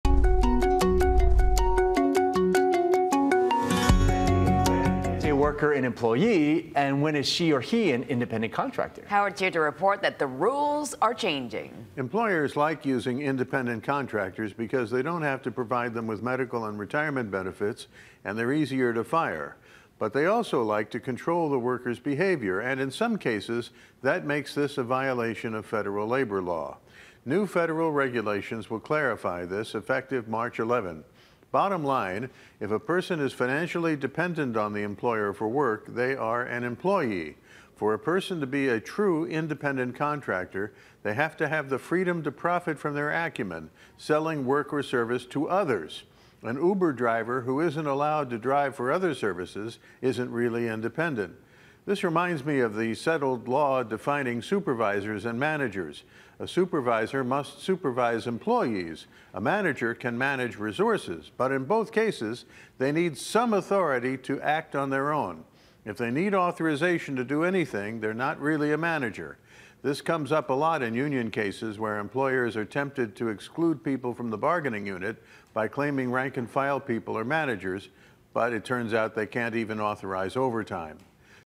Business Report: New federal regulations on independent contractors on the horizon
When is a worker an employee and when is she/he an independent contractor? Howard Dicus reports the rules are changing.